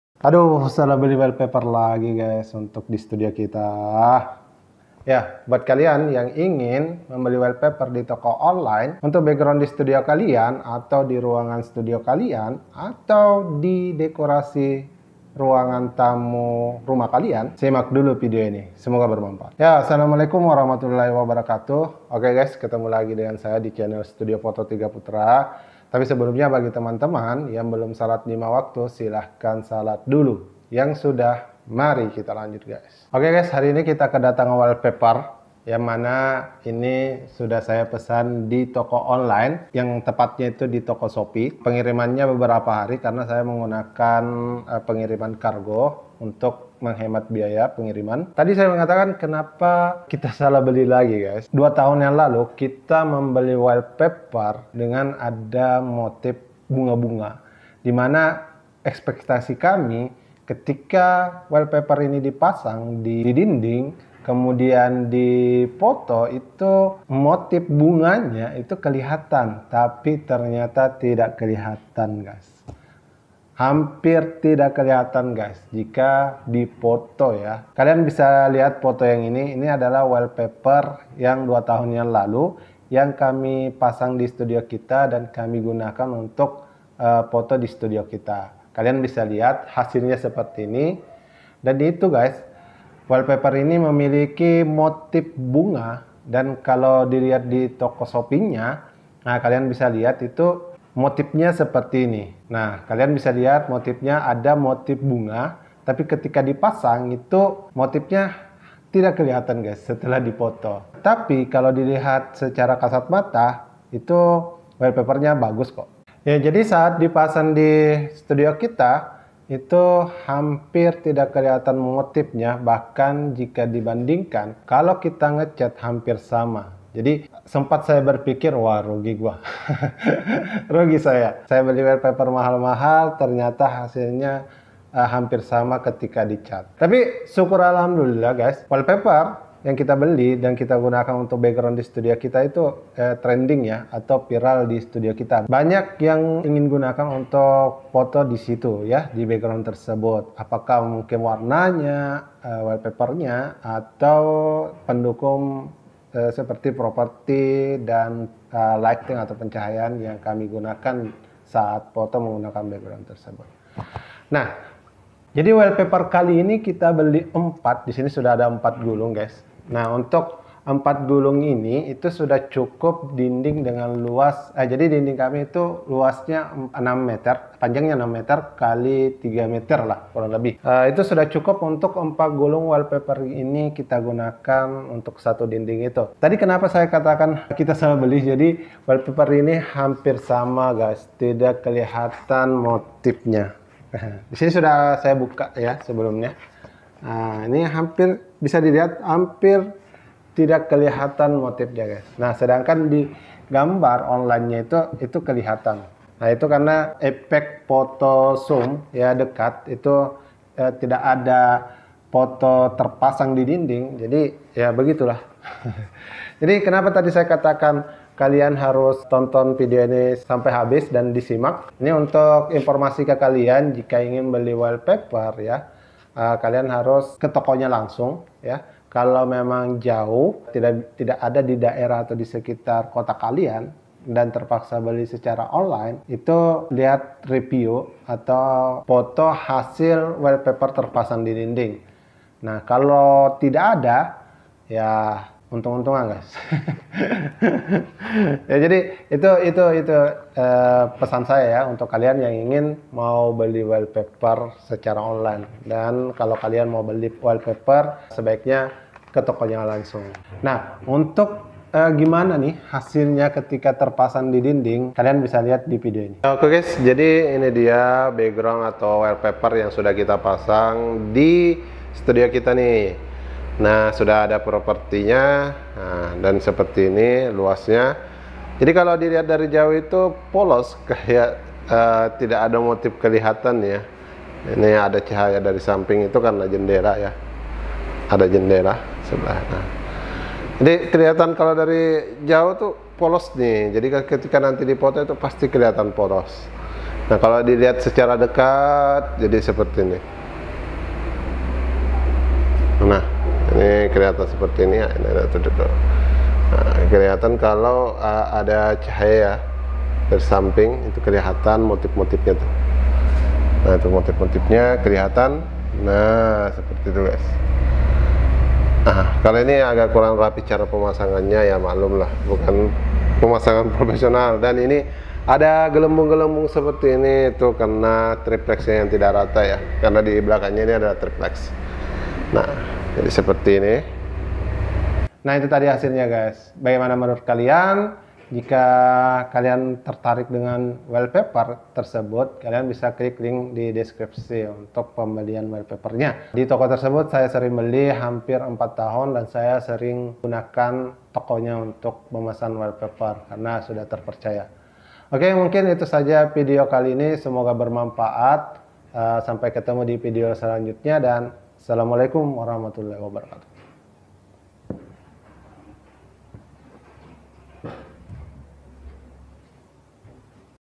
wallpaper untuk background studio
setiap tahun kami selalu beli wallpaper untuk mengganti wallpapaer di studio kami supaya ada nuansa baru di studio foto kami tapi kali ini kami beli wallpaper yang motifnya tidak kelihatan dan untuk hasilnya kalian bisa liat di video ini

playlist / video yang di pisah sesaui kategori
klik d sini untuk